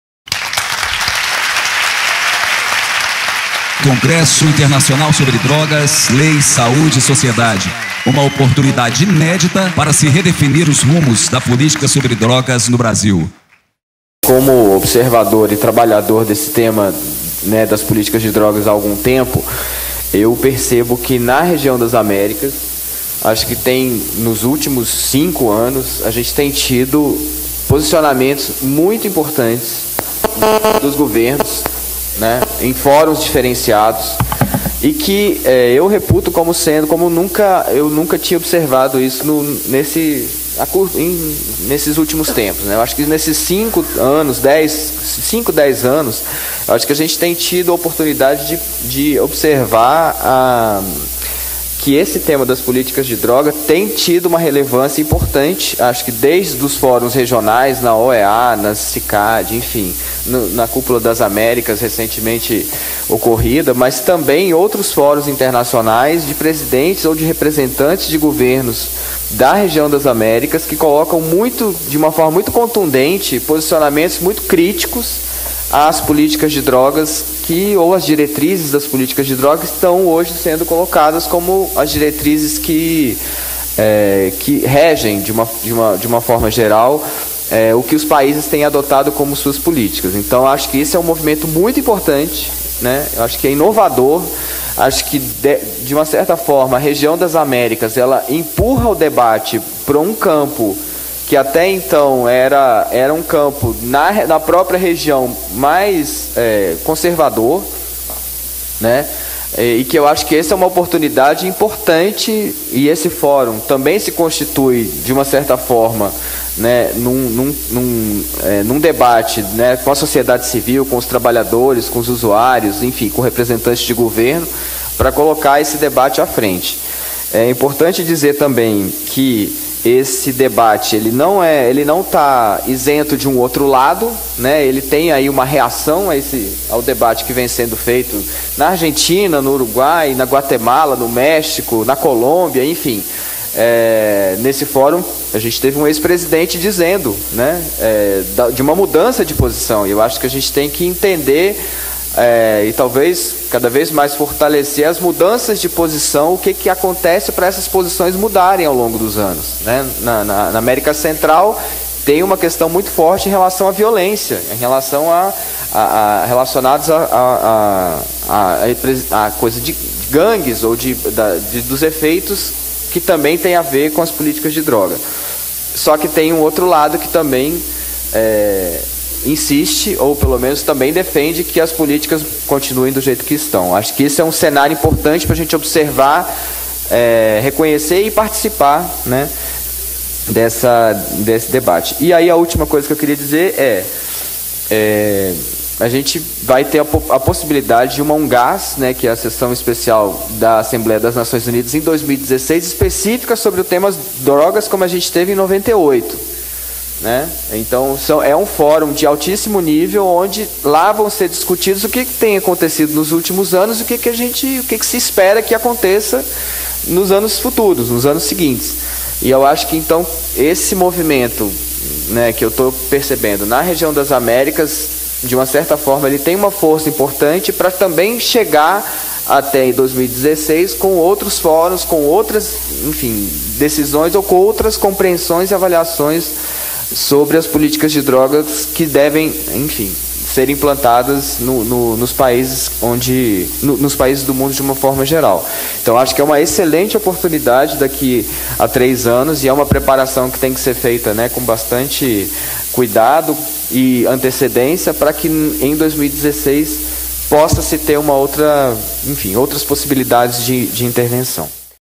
FRANCISCO CORDEIRO: Drogas na América Latina - Desafios e Alternativas - CID2013 - 5Maio - 2(Pt4)
Mesa Redonda: Drogas na América Latina - Desafios e Alternativas